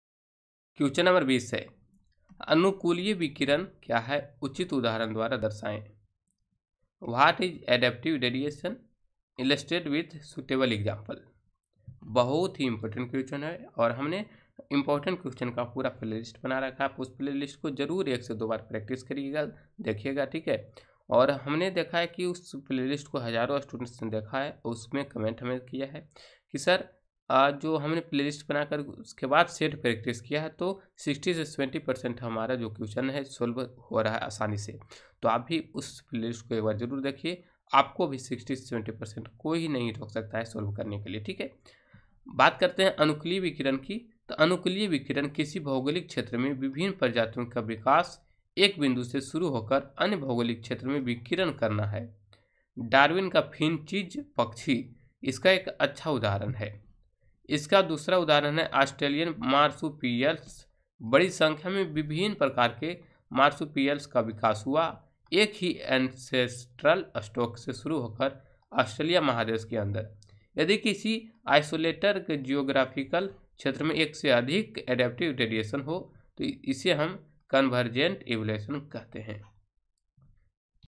अनुकूलिय विकिरण क्या है? उचित उदाहरण द्वारा दर्शायें। What is adaptive radiation? Illustrate with s
अनुकूलिय विकिरण क्या है? उचित उदाहरण द्वारा दर्शायें। What is adaptive radiation? Illustrate with suitable example. | anukooliy vikiran kya hai? uchit udaaharan dvaara darshaayen.